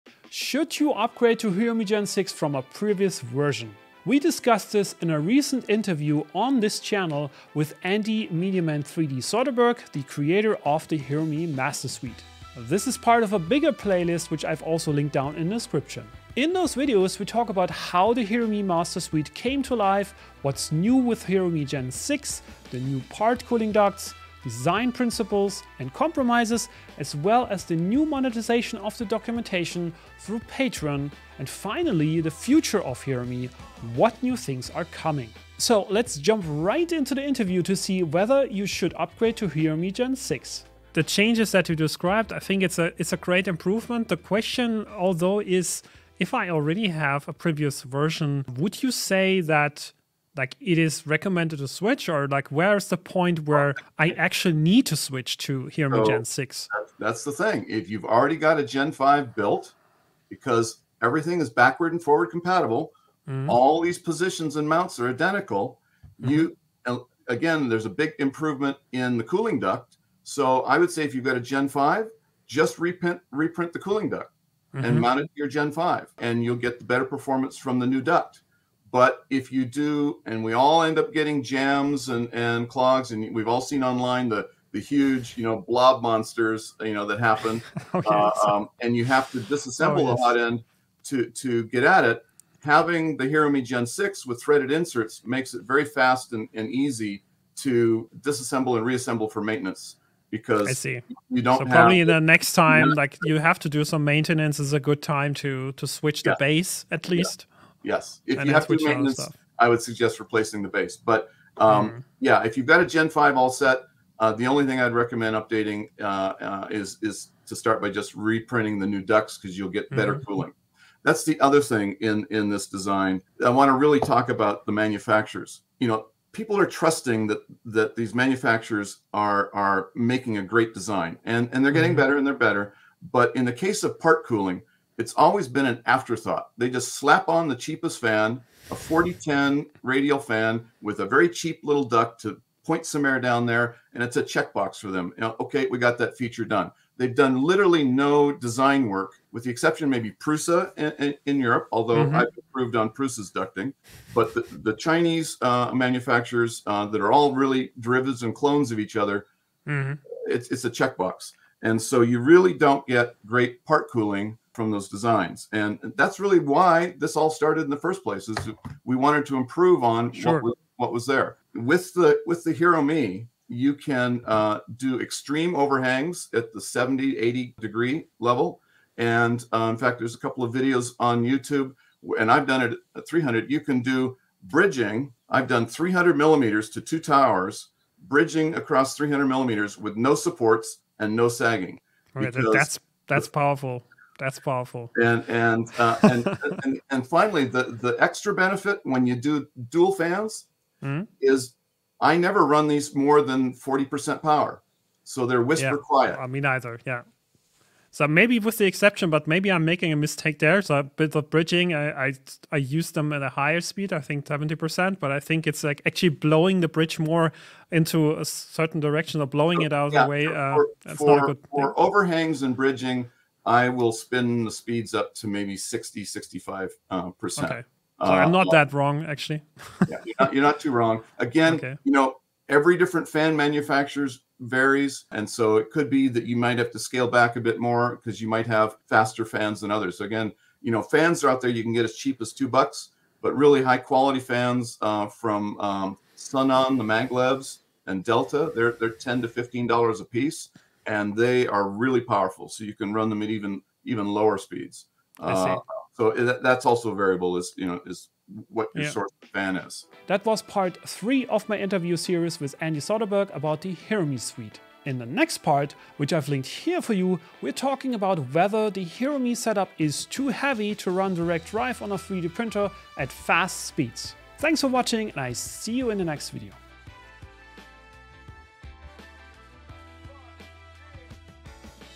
Why upgrade to Hero Me Gen 6? - With Andy MediaMan3D Soderberg
Should you upgrade to Hero Me Gen 6 from a previous version?

We discussed this in a recent interview on this channel with Andy MediaMan3D Soderberg, the creator of the Hero Me Master Suite.  This is part of a bigger playlist, which I have also linked down below.

In those videos, we talk about how the Hero Me Suite came to life, what’s new with Hero Me Gen 6, the new part cooling ducts, design principles and compromises as well as the new monetization of the documentation through Patreon and finally the future of Hero Me - what new things are coming.